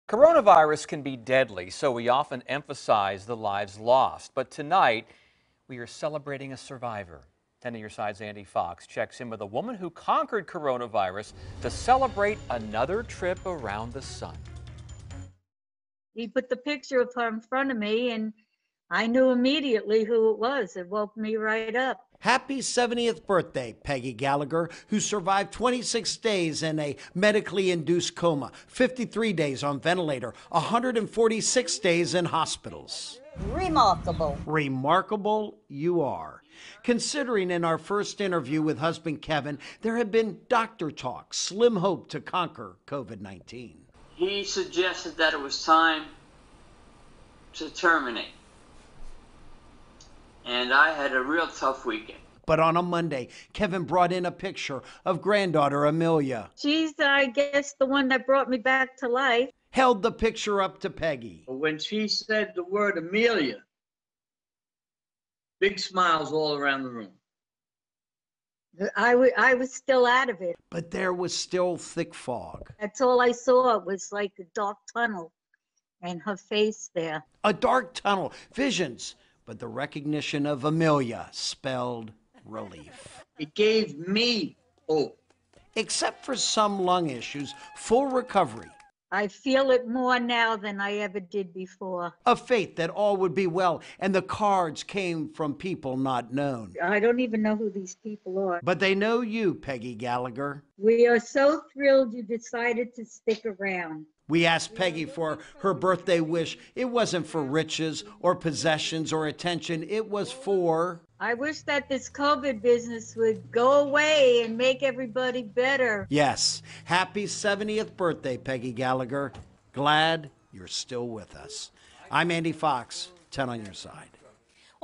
Mathews woman turns 70 after defying all odds and beating COVID-19
WAVY News 10's Andy Fox reports.